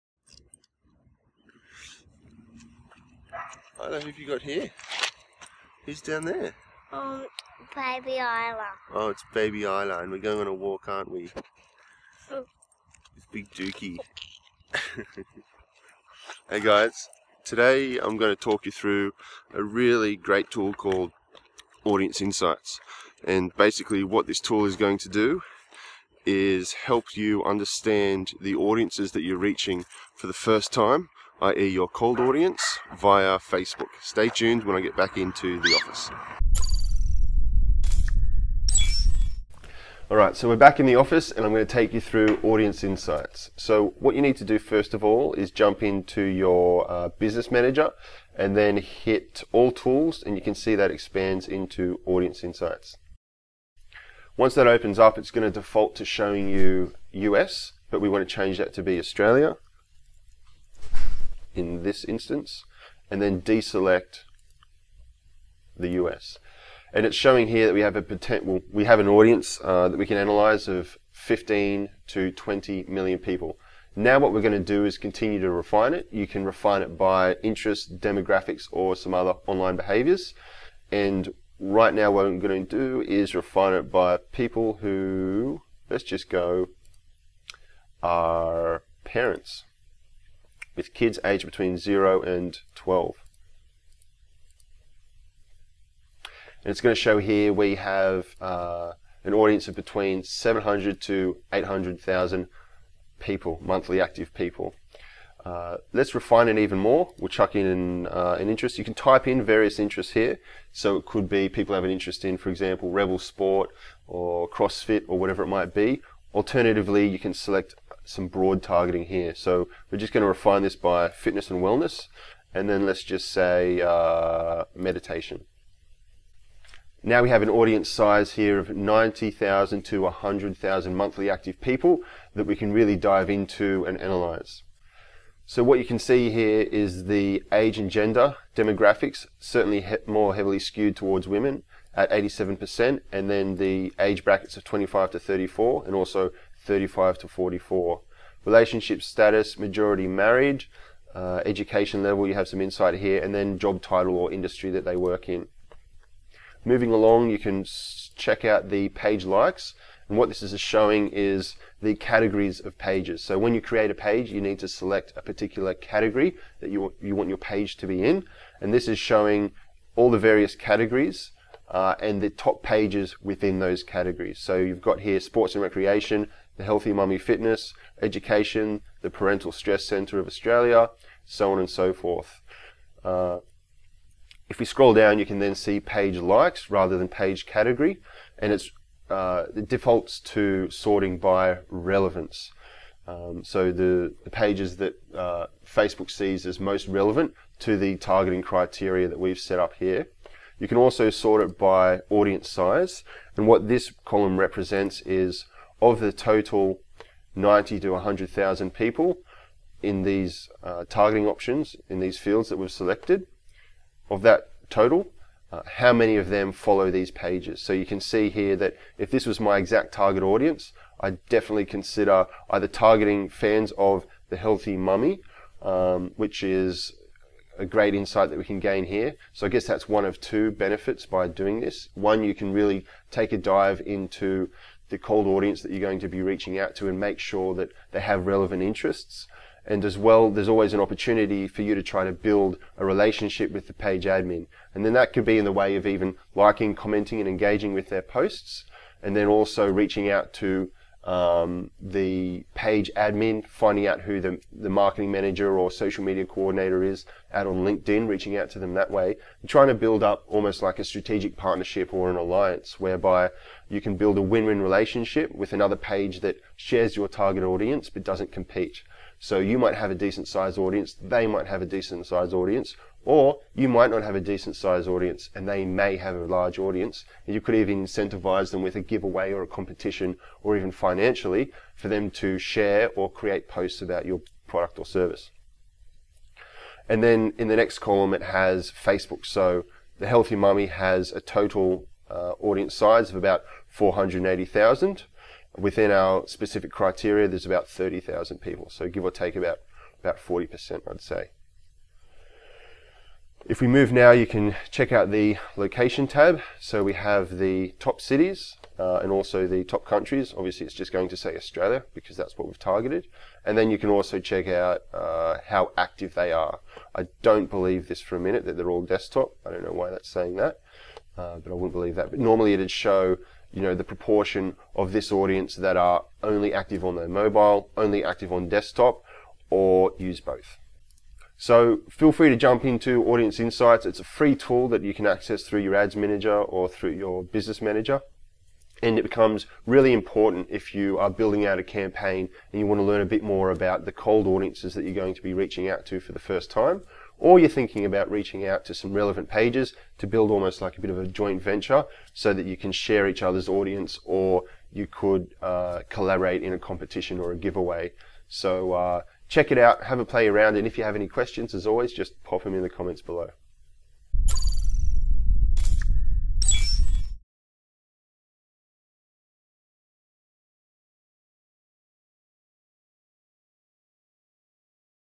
Facebook Audience Insights
Understand your target audience like never before with Facebook's 'Audience Insights'.

Audience Insights is a great way to get to know your Cold Audience on a deeper level and target fans of competitors' pages that make up a significant part of your audience.

If that page isn’t a competitor then it’s a great opportunity to approach them for a strategic partnership, which I’ll explain in more detail in this video.

Boost the results of your current social media campaigns with a free 30-minute Strategy Session.

Cheers, Perry.